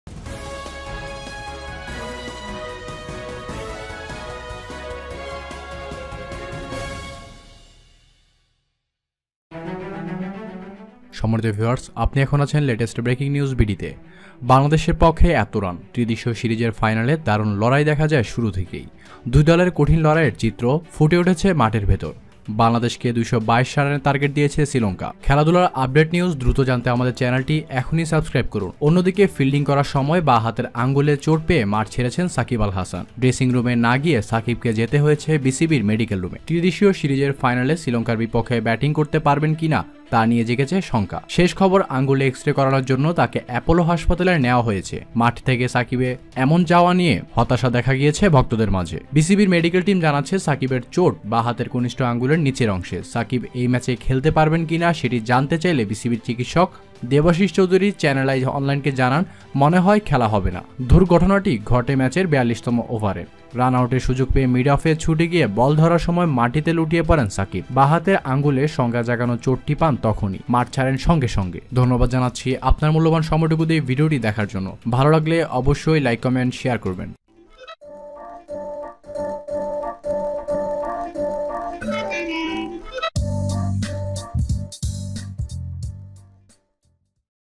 বাংলাদেশকে বিশাল রানের টার্গেট দিলো শ্রীলঙ্কা / সর্বনাশ! হাসপাতালে সাকিব- Ban VS SL Final Match
Shakib Al Hasan left the field with his left hand fingers while fielding. Shakib did not go to the dressing room to go to BCB medical room.

Whether to bat against Sri Lanka in the tri-series final; Wondered about it Last news, he is being taken to Apollo Hospital for X-rays on the finger. From the field of hospital Shakib, who will bat?